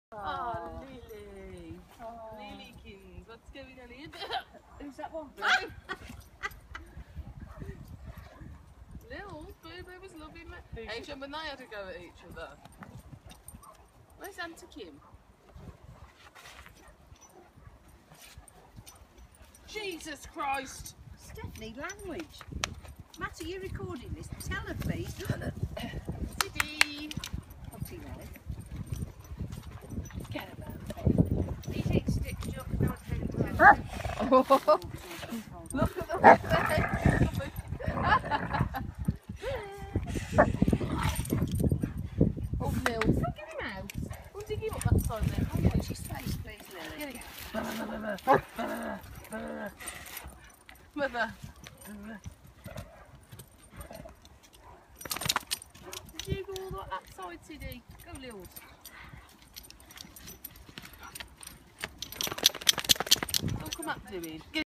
Great Dane Digging Session
This is normally Sid's hole but Lilly tries to muscle in. Sid doesn't like it at all. Play nice kids